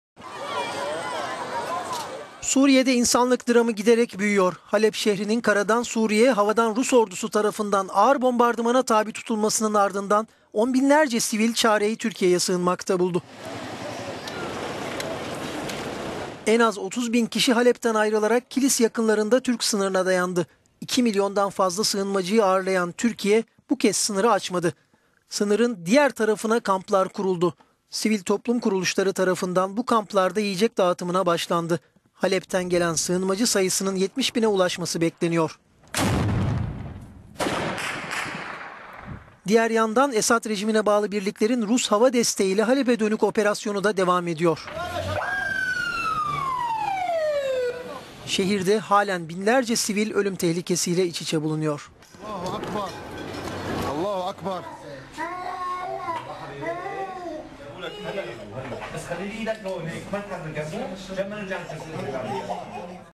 Halepten kaçan siviller sınırda, sınır bu kez kapalı
Suriye'de insanlık dramı giderek büyüyor. Halep şehrinin karadan Suriye havadan Rus ordusu tarafından ağır bombardımana tabi tutulmasının ardından on binlerce kişi çareyi Türkiye'ye sığınmakta buldu.

En az 30 bin kişi Halep'ten ayrılarak Kilis yakınlarında Türk sınırına dayandı. 2 milyondan fazla sığınmacıyı ağırlayan Türkiye bu kez sınırı açmazken Suriye tarafına kamplar kuruldu. Bu kamplarda yiyecek dağıtımına başlandı. Halepten gelen sığınmacı sayısının 70 bine ulaşması bekleniyor.